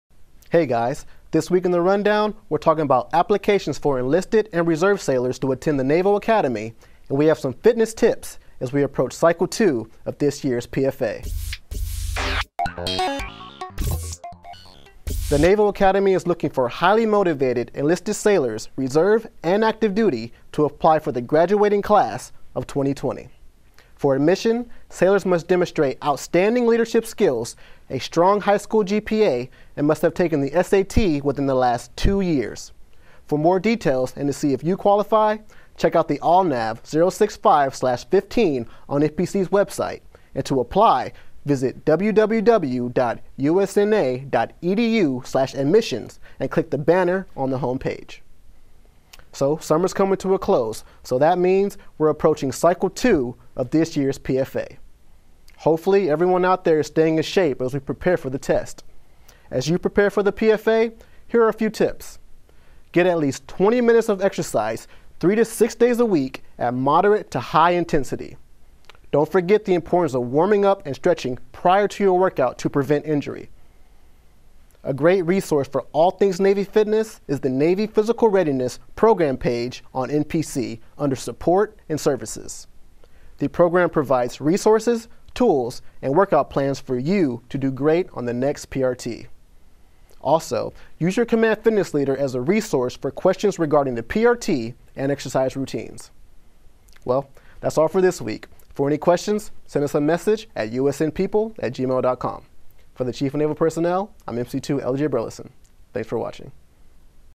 APPLICATIONS TO USNA AND PRT RESOURCES
THIS WEEK IN THE RUNDOWN WE’RE TALKING ABOUT APPLICATIONS FOR ENLISTED AND RESERVE SAILORS TO ATTEND THE NAVAL ACADEMY AND WE HAVE SOME FITNESS TIPS AS WE APPROACH CYCLE TWO OF THIS YEAR’S P-F-A.